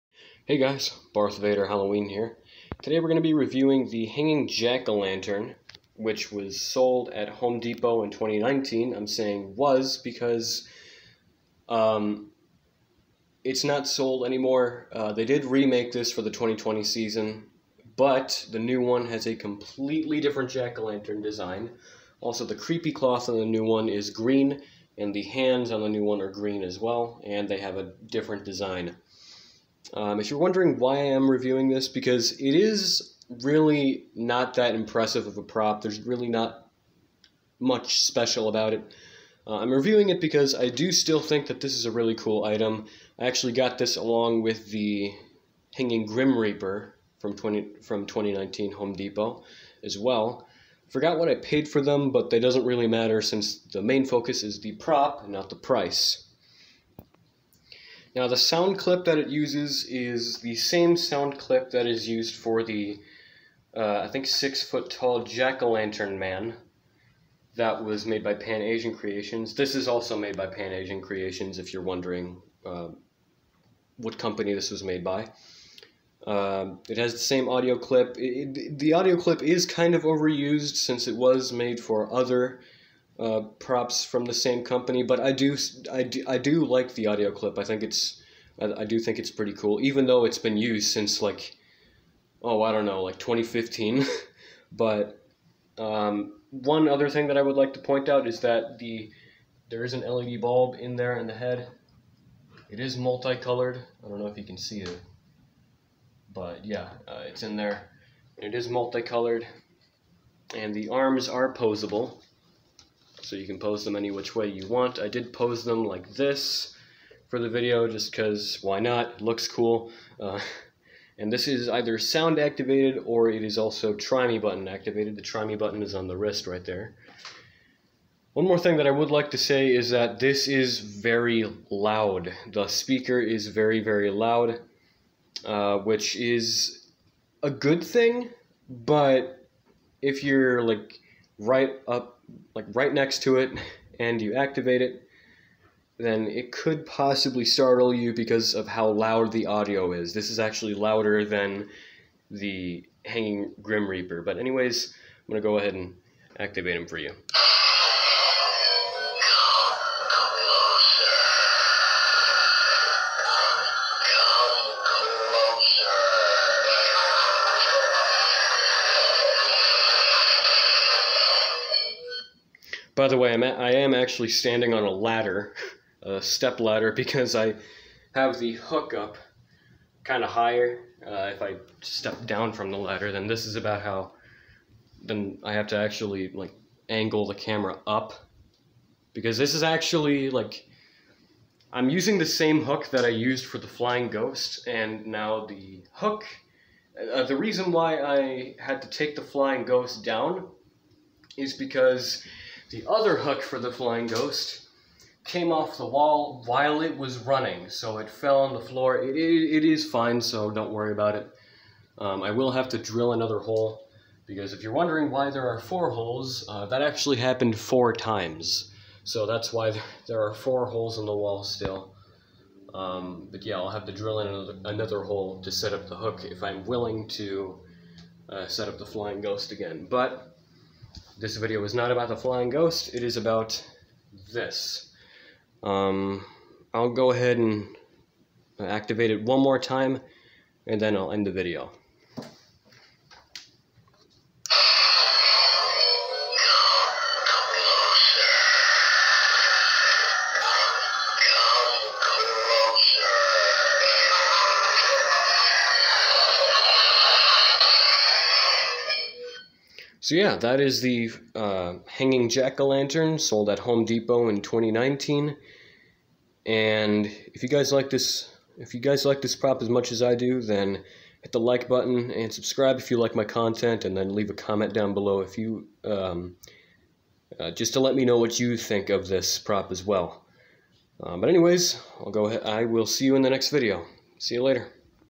PAC 2019 hanging Jack-o'-lantern Review
This is just a short review of the Hanging Jack-o'-lantern prop that was sold at home depot in 2019 and made by Pan Asian Creations.